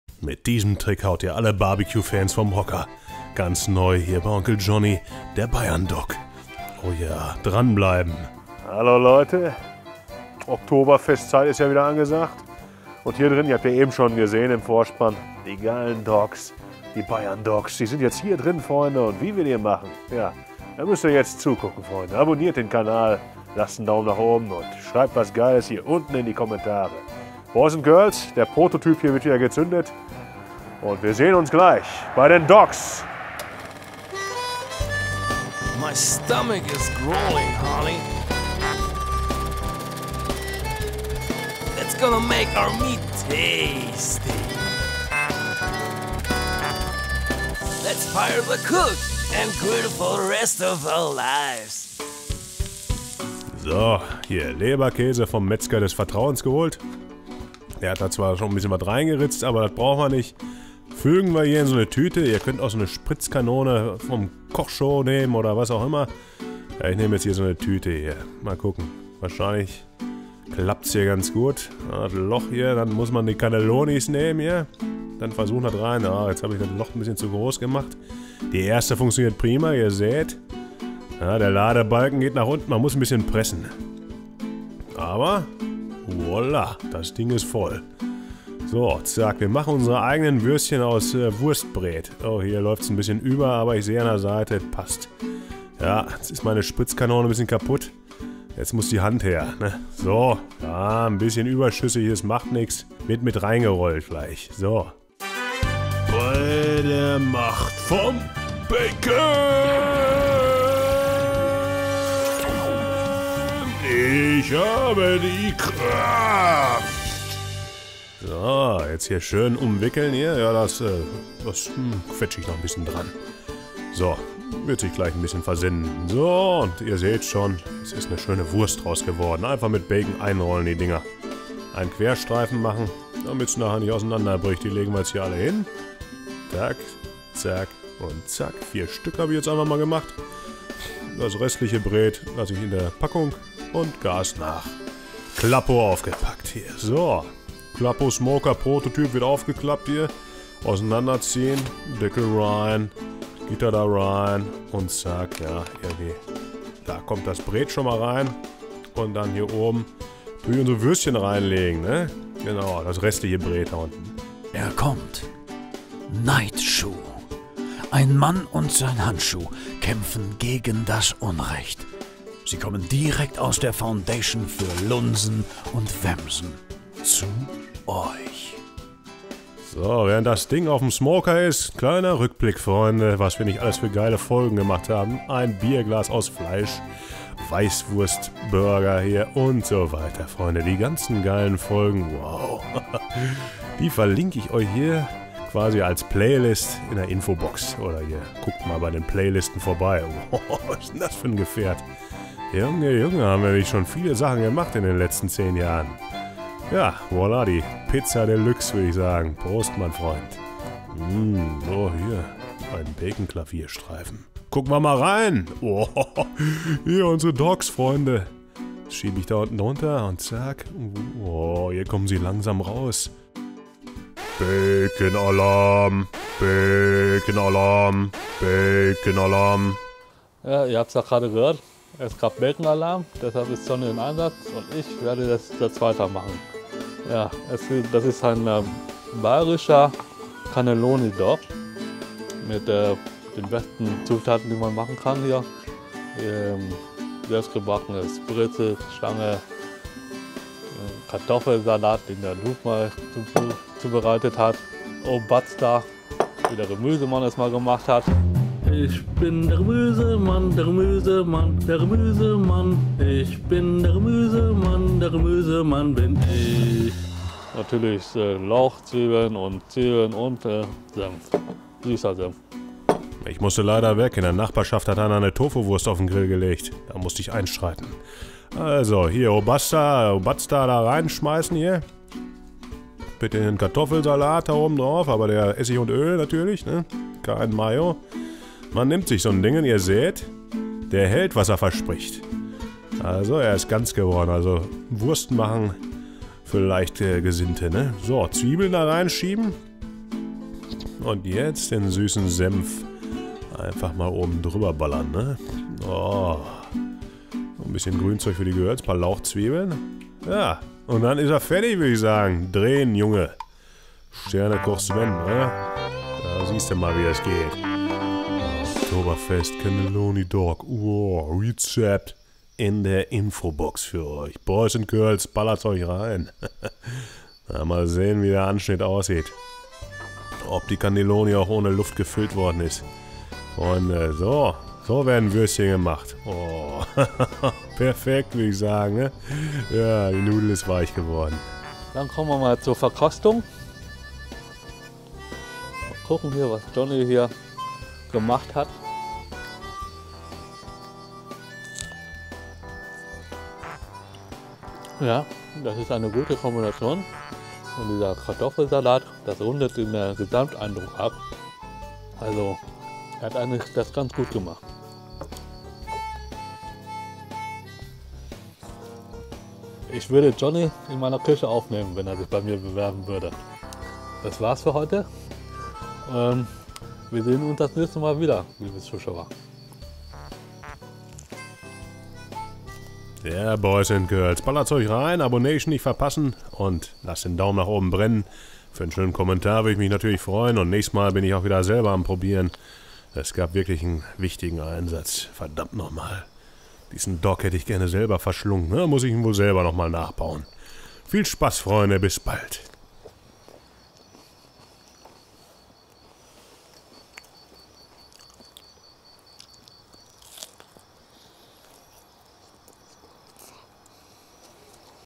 O'zapft is! Der Bayern Dog | Die Grillshow 516
Die Burger-Presse, der Klappo, der Striker und noch viel mehr heißer BBQ-Stuff!

O'zapft is, Freunde! Endlich ist wieder Wiesn-Zeit! Und natürlich hauen wir wieder ein Oktoberfest Special der Spitzenklasse raus, Freunde! Heute gibt es den Bayern-Dog. Mit einer Wurst, die aus gefüllten Cannelloni mit Bayerischem Leberkäs besteht. Unfassbar reinballernswert, Freunde! Ein Prosit! Ab in Hals!

Das braucht ihr (für 4 Dogs):
- 8 Cannelloni
- ca. 500 g Leberkäs
- ca. 400 g Bacon-Streifen

zum Belegen:
- frische Laugenstangen
- Obazda
- Bayerischer Kartoffelsalat
- süßen Senf
 - rote Zwiebeln
- Frühlingszwiebeln

HÜÜÜÜAHHHHH!!!
euer Johnny